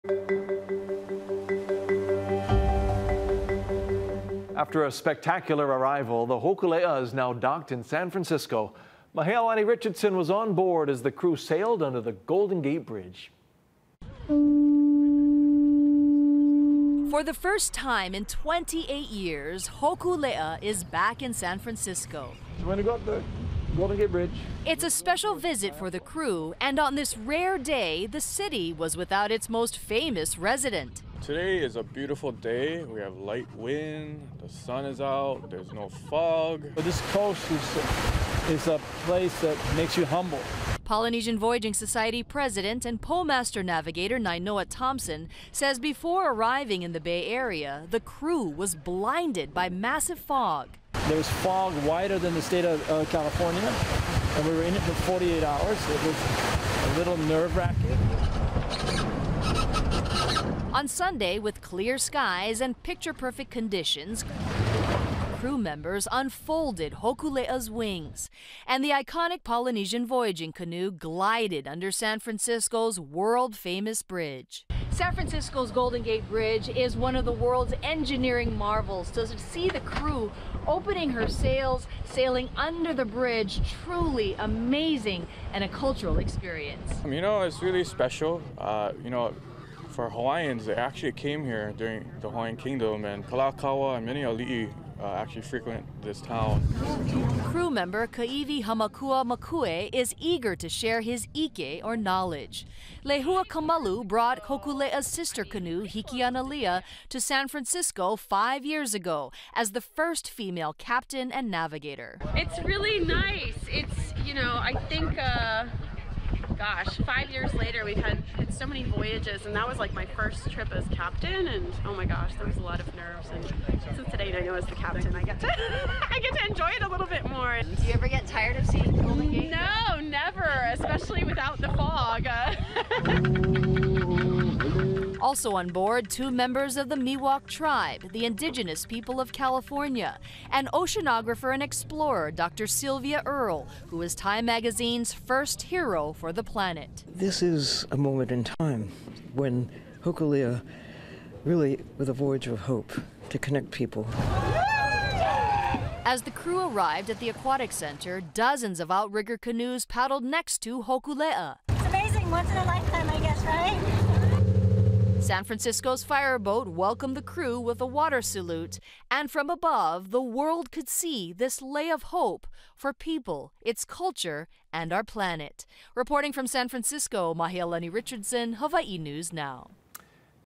The fog (temporarily) lifts in San Francisco for Hokulea’s much-anticipated arrival
For the first time in nearly 30 years years, Hokulea is back in San Francisco.